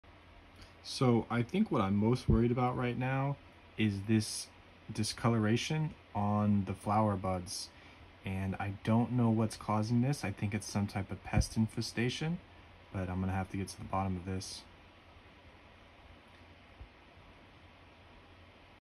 Black discoloration on pepper flowers. Pests? Neem oil residue?
Today I noticed this - it is a black discoloration. Many flowers on this plant have this, and it appears on the stem/head of the flower. It almost looks like scarring and it does not wipe away off the plant.

There ARE pests on this plant. They seem to be very small black pests and are visible in some of the grow logs. Currently Azamax & Neem Oil have been applied in the last seven days.